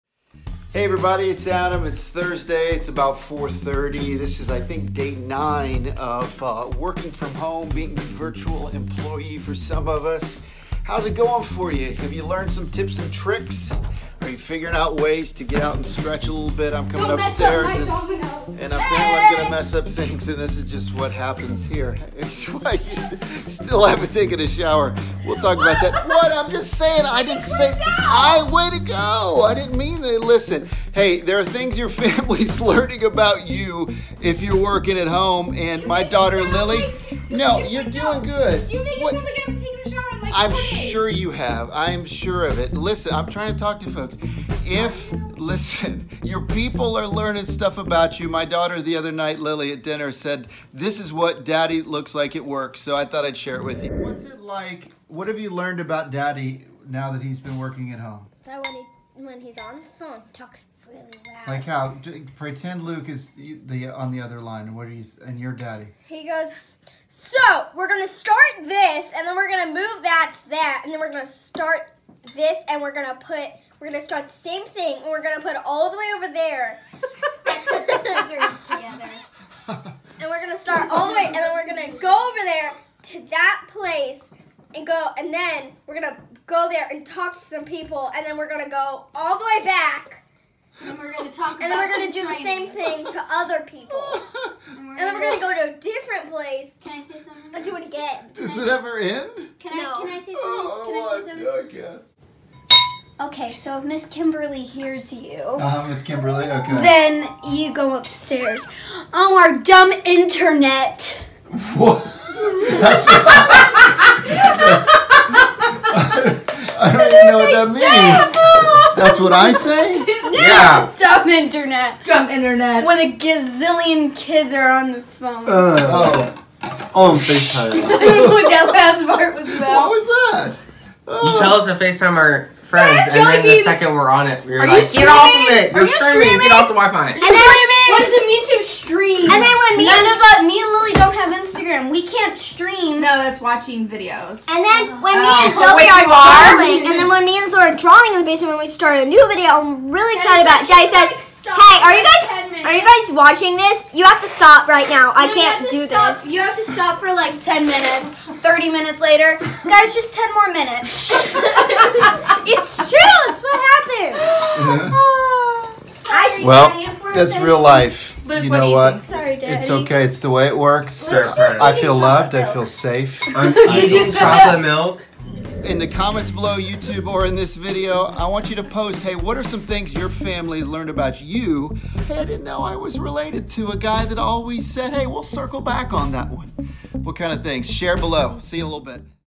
Things I've learned about the "work from home" me.
Your family, friends, and roommates may know the "home" YOU, but do they know the "work from home" YOU? Post in the comments below something new, funny, or surprising you've learned about yourself from others while working from home.